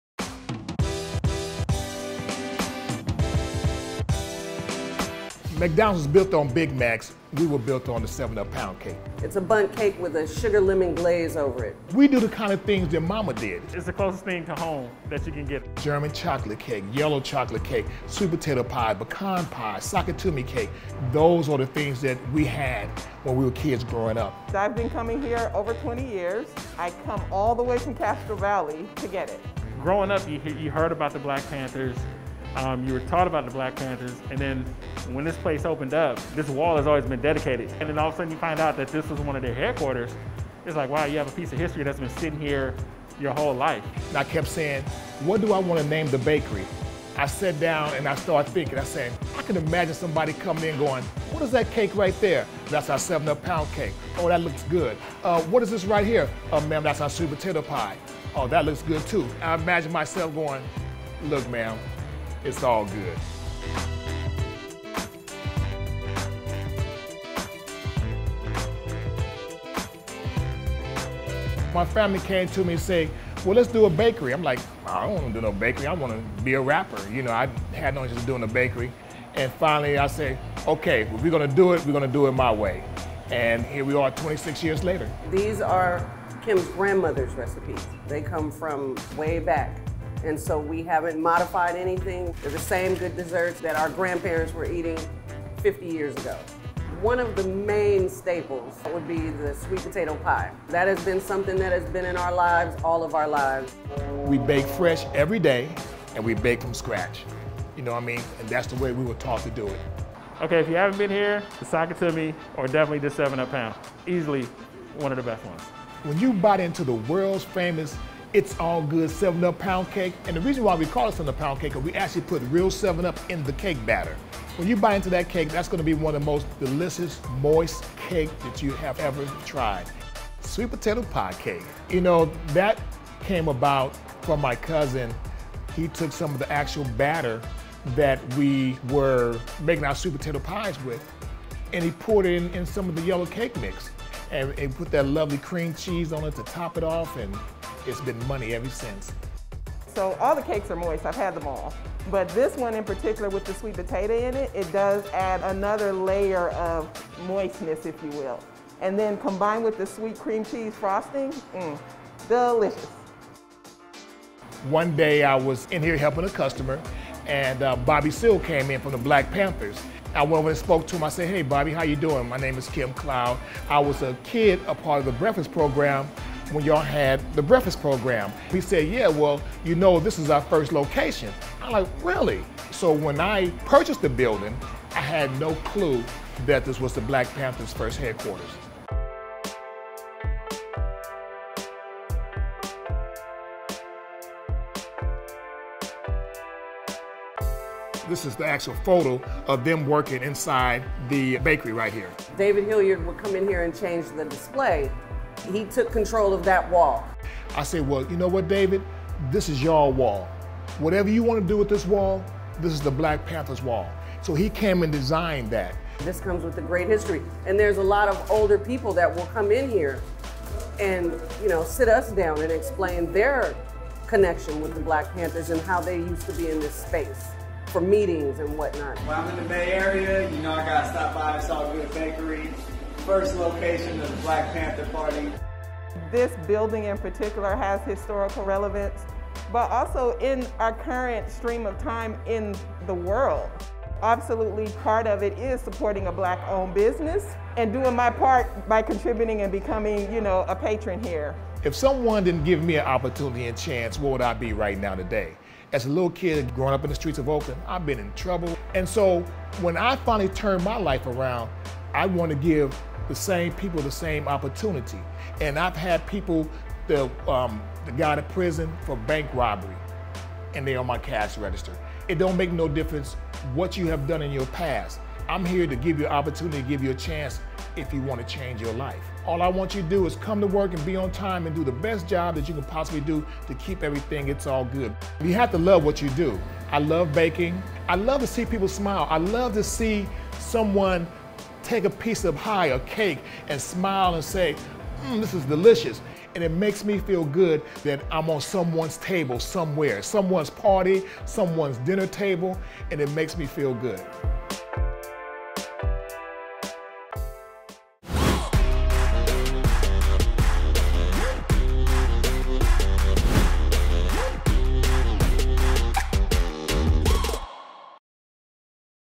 This bakery was the Black Panthers' first headquarters | Bite Size
Serving delectable treats made from scratch, it's all good at this iconic Oakland, California bakery-hence the name!It's All Good Bakery's popular dessert recipes are derived from Owner Kim Cloud's family cookbook and provide a slice of home for customers. 'We do the kind of things your mama did,' said Kim Cloud. While the bakery is known for its signature 7-Up Cake, Sweet Potato Pie Cake, Sweet Potato Pie, and more, the spot also serves as a historic landmark. The building that houses It's All Good Bakery was the first headquarters for the Black Panther Party. Inside, there is a tribute wall dedicated to the Black Panthers, which features milestone photos. David Hilliard, who was the Black Panthers' Ch...